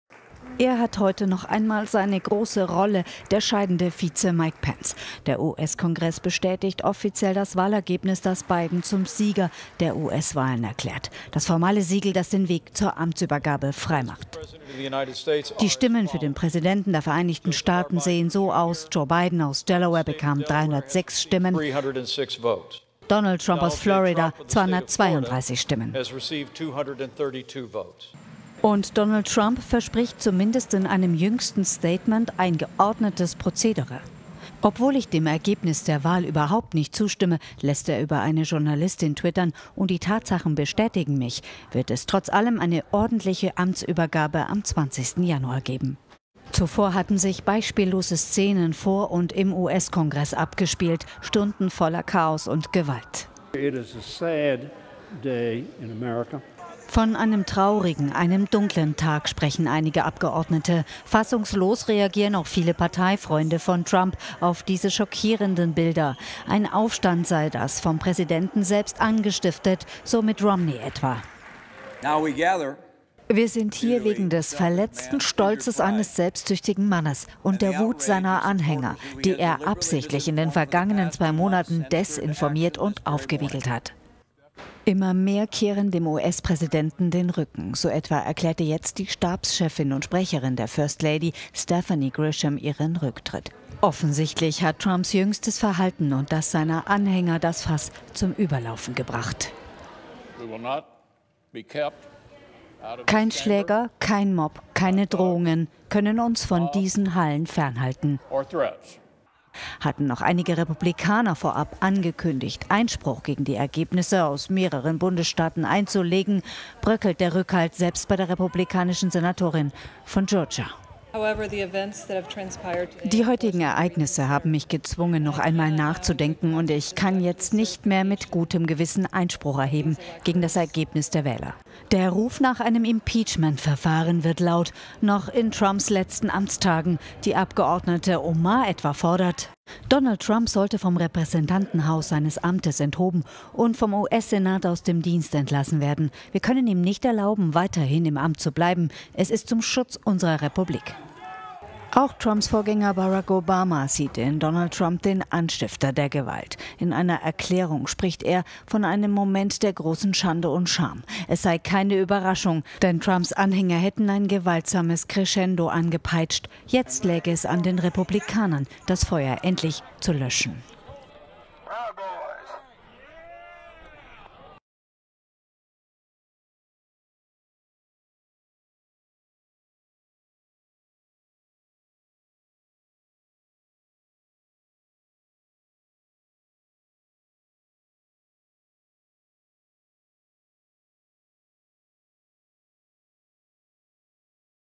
MÖGLICHE ABSETZUNG: Kabinettmitglieder erwägen DONALD TRUMP seines Amtes zu entheben
Hochrangige Mitglieder der scheidenden US-Regierung haben laut übereinstimmenden Medienberichten über eine mögliche Absetzung von Präsident Donald Trump durch sein eigenes Kabinett beraten. Nach Informationen der US-Sender CNN, CBS und ABC sollen sich diese Überlegungen auf einen Zusatzartikel zur US-Verfassung gestützt haben, der die Entmachtung des Präsidenten durch das Kabinett grundsätzlich erlaubt.

Auch Rufe nach einem erneuten Amtsenthebungsverfahren nehmen nach den Ausschreitungen zu. Die demokratische Kongressabgeordnete Ilhan Omar teilte am Mittwoch auf Twitter mit, sie fertige bereits Artikel zur Anklageerhebung an. „Wir können nicht zulassen, dass er im Amt bleiben, es ist eine Frage der Erhaltung unserer Republik, und wir müssen unseren Eid erfüllen.“ Ihre Kollegin Carolyn Bourdeaux schloss sich der Forderung an und machte Trump persönlich für den Angriff verantwortlich.